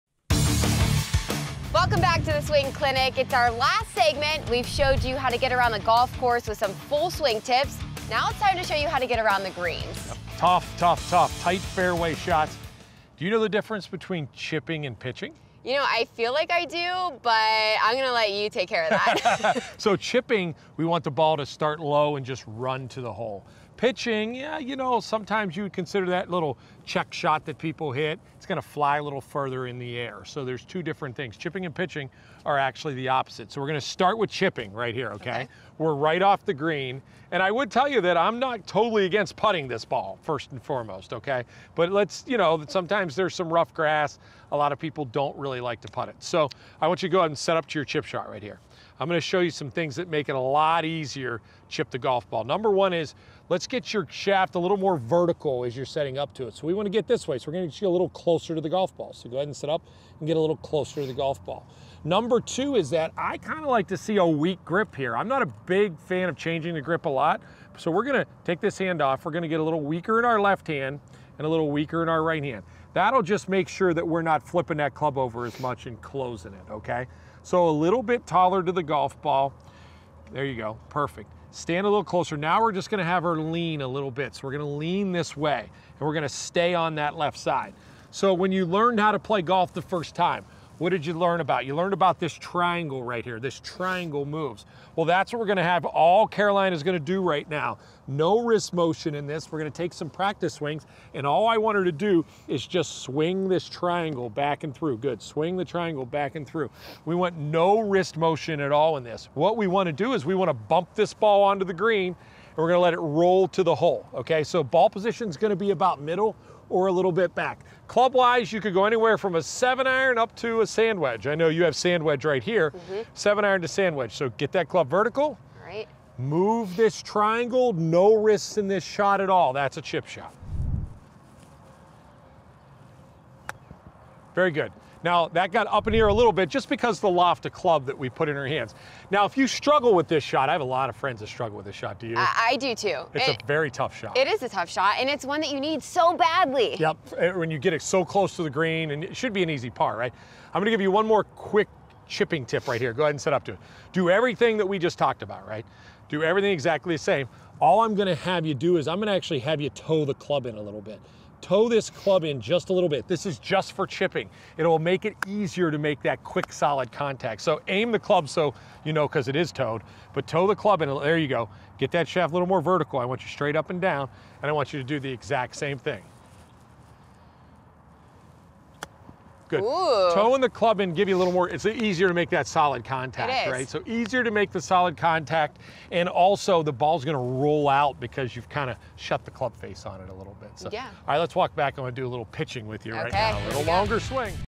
Swing Clinic: Chipping Tip
During this segment of the Swing Clinic, Jimmy Hanlin shows Caroline Collins some tips on chipping the ball onto the green at the Little Mountain Country Club in Concord, Ohio.